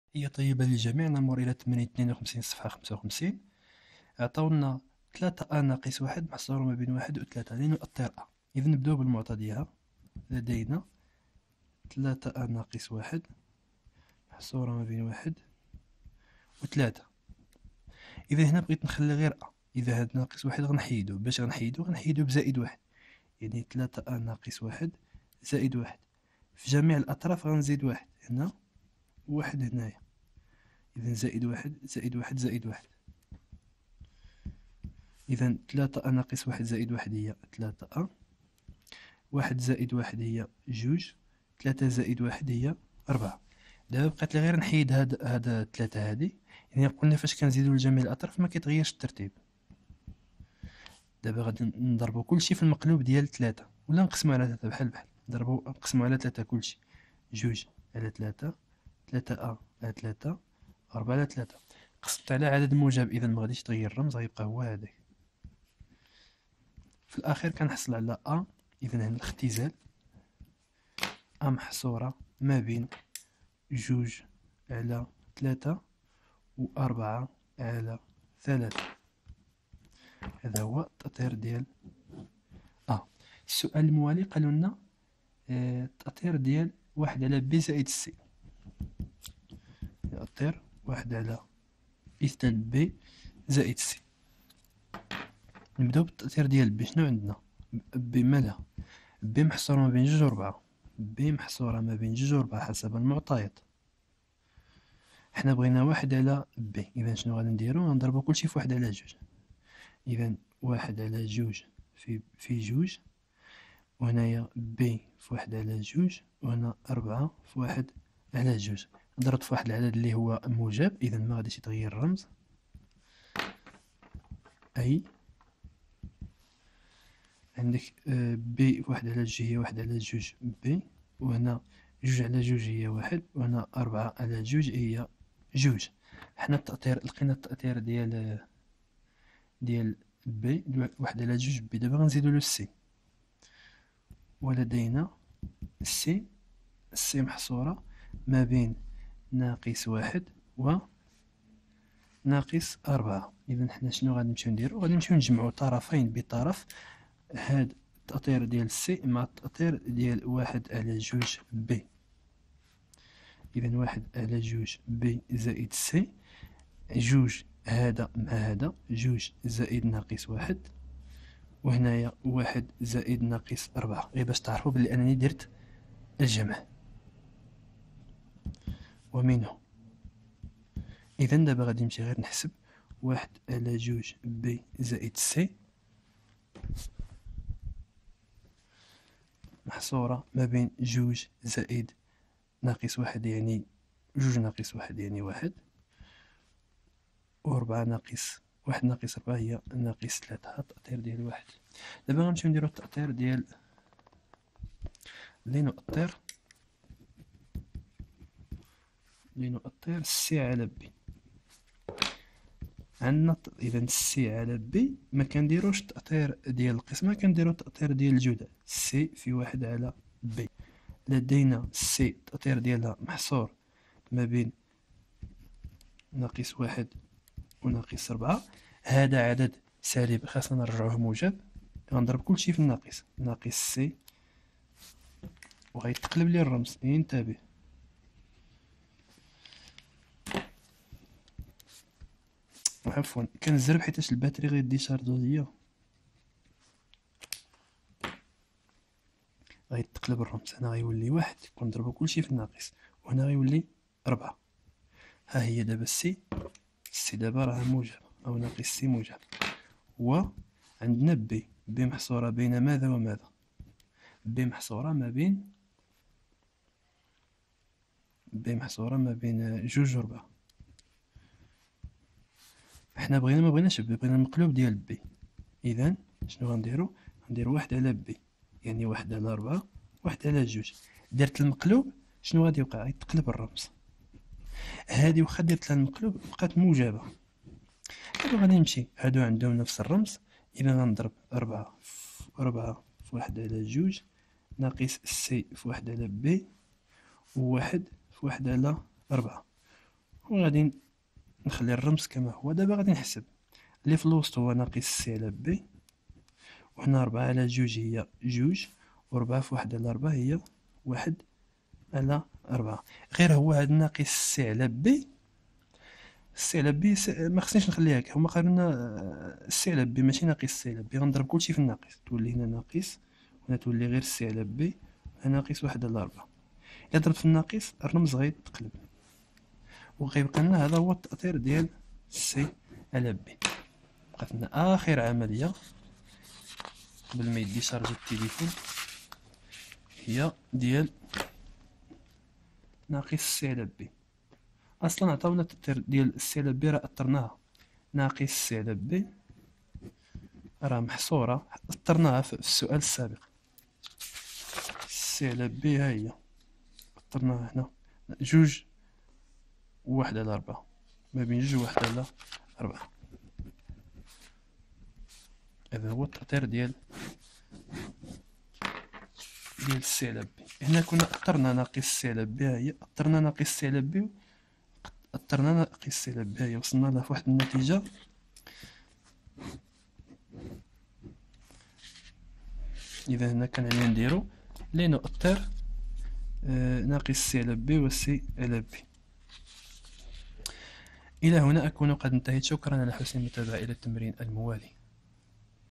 التمرين 52ص 55 من كتاب المفيد في الرياضيات لمستوى الثالثة إعدادي
ترتيب العمليات على الاعداد النسبيه للصف الثاني متوسط حل تمارين,